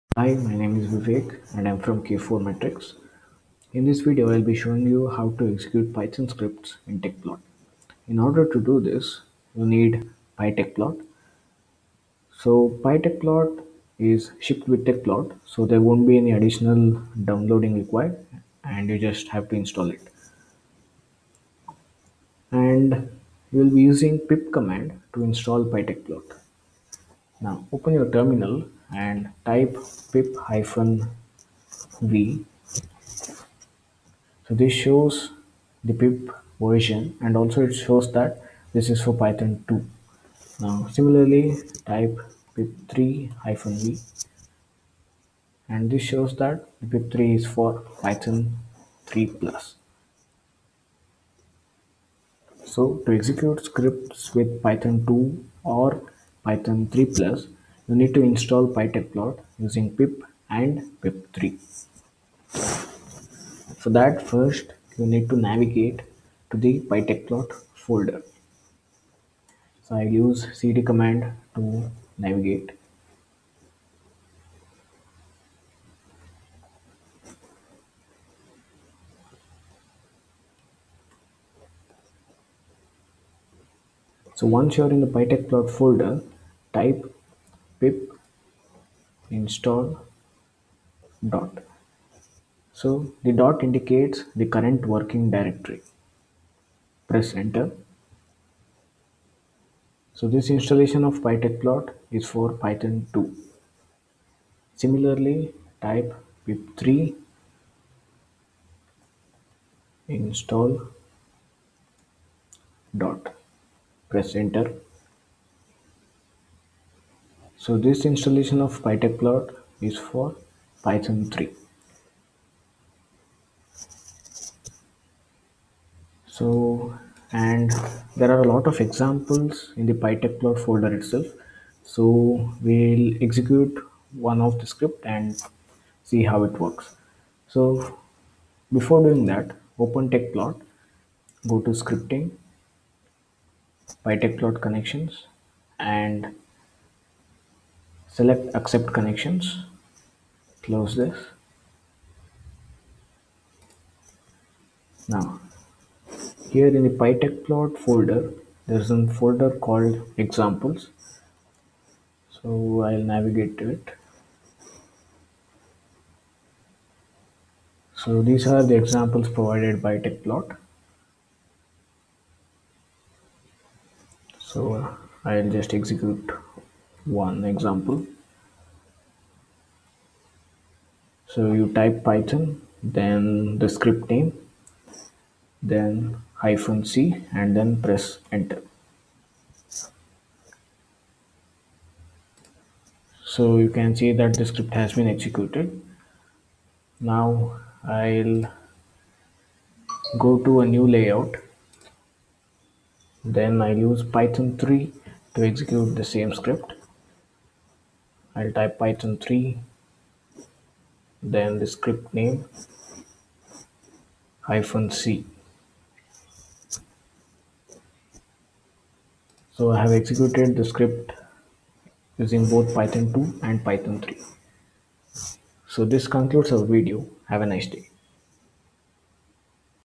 Python Scripts with Tecplot
Learn how to install PyTecplot using pip, enable Python Connections and test your setup.